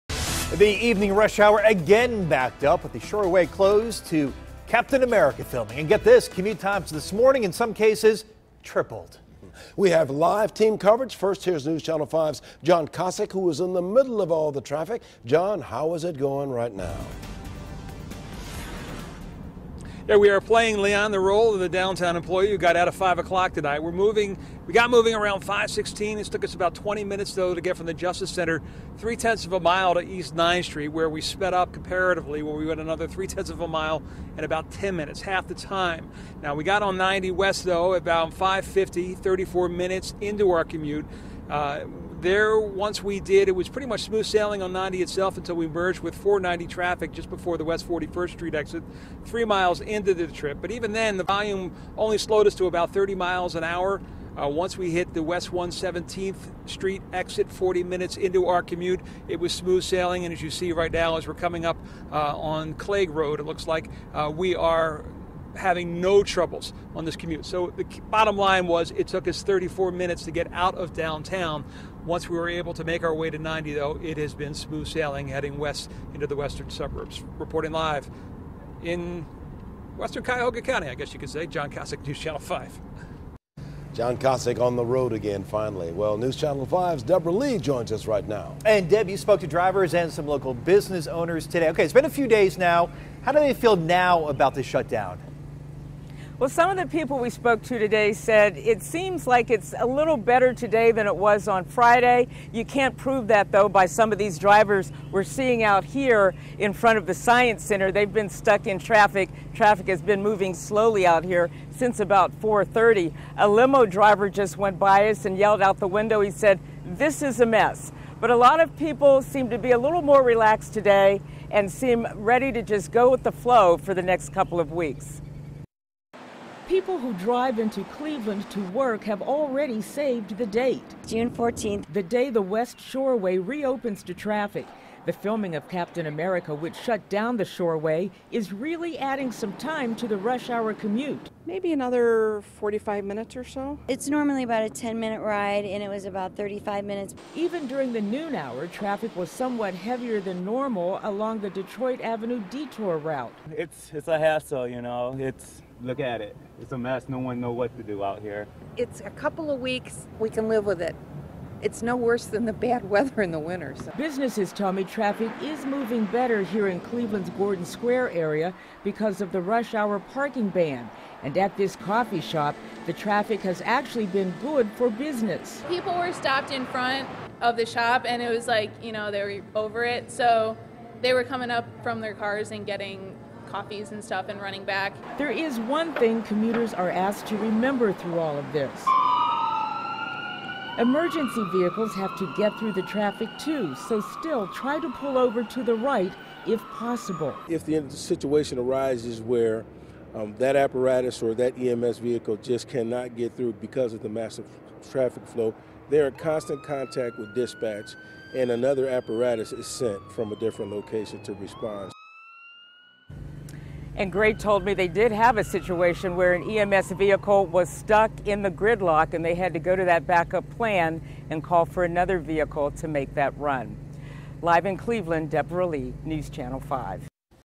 6pm: Shoreway closure still a hassle for some
Cleveland commuters said getting to work was still a hassle Monday because of the West Shoreway shutdown.